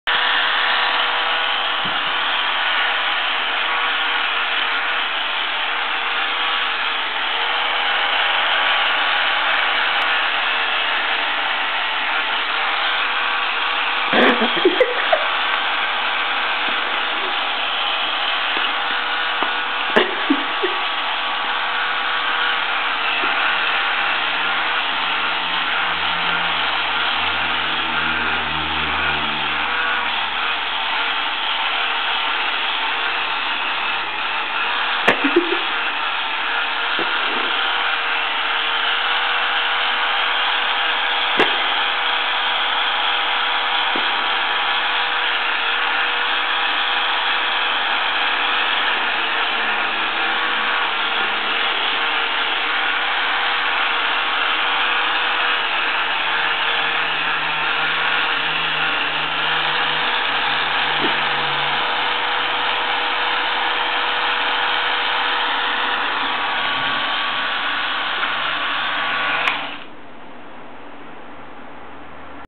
Slayne protecting his mummy from the epilator
Slayne protecting his mummy from the monster epilator